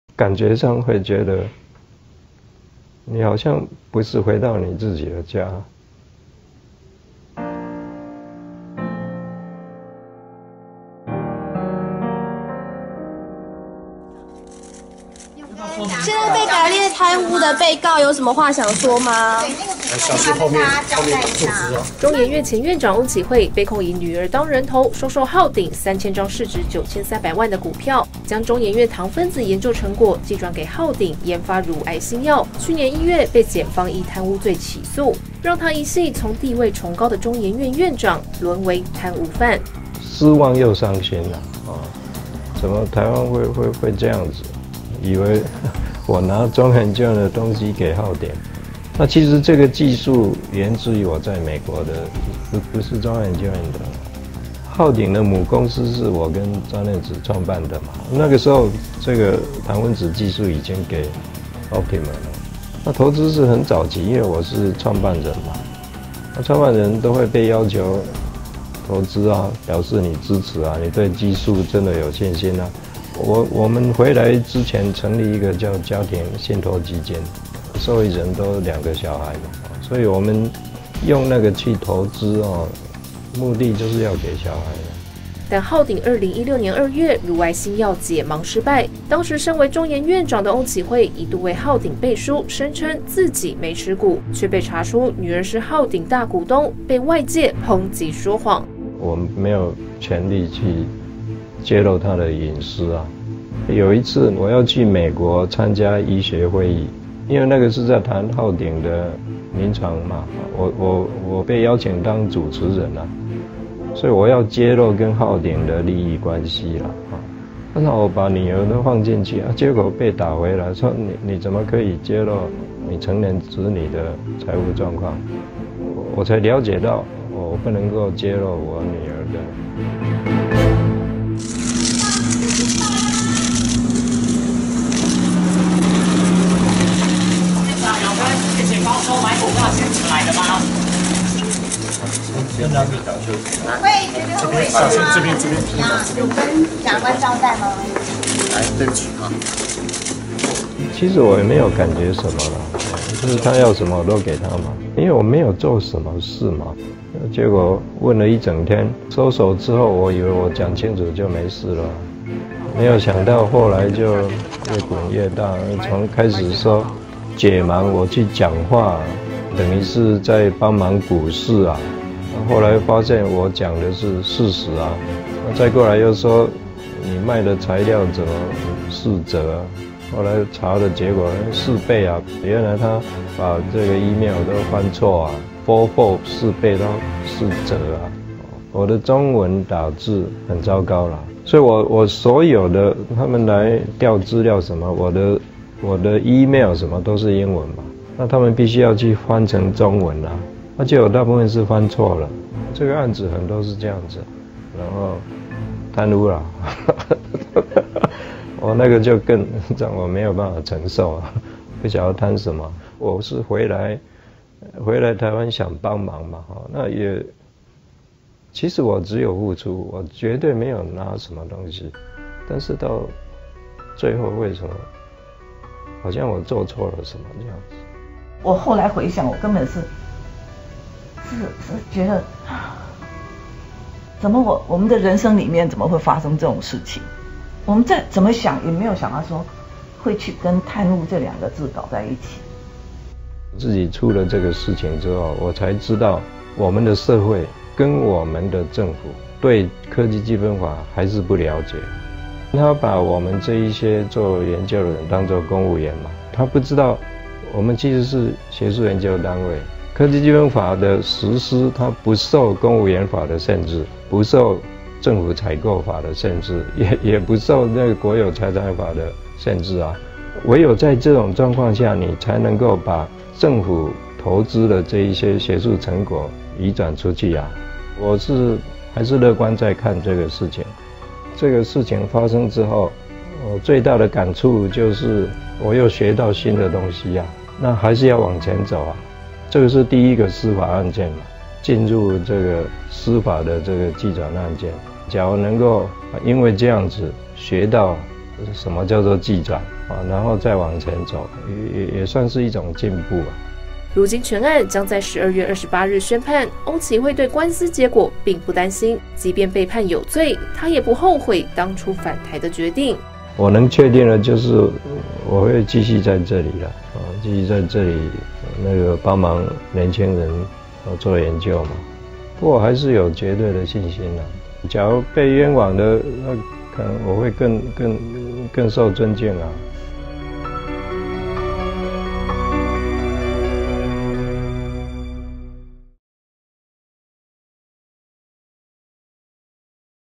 【蘋果人物1】深陷浩鼎案 翁啟惠哽咽「我做錯什麼？」 | 台灣蘋果日報
發表過700篇論文、擁有100多項專利的翁啟惠，是全球首位成功研發「一鍋式合成法」及以酵素技術合成大量複雜多糖物的科學家，對許多疾病新藥開發貢獻良多，2006年回台接任中研院長，卻在2016年因浩鼎案淪為貪污犯，失去院長一職，在捲入浩鼎案的903個日子中，他始終冷對外界指控與官司，然而就在接受《蘋果》專訪這天，他罕見地哽咽了，他失落的說：「我回台灣是想幫忙，只有付出，但是到最後卻變成我做錯了什麼，感覺不是回到自己的家……」，為什麼一心貢獻，卻落得這步田地，讓他沉痛無語。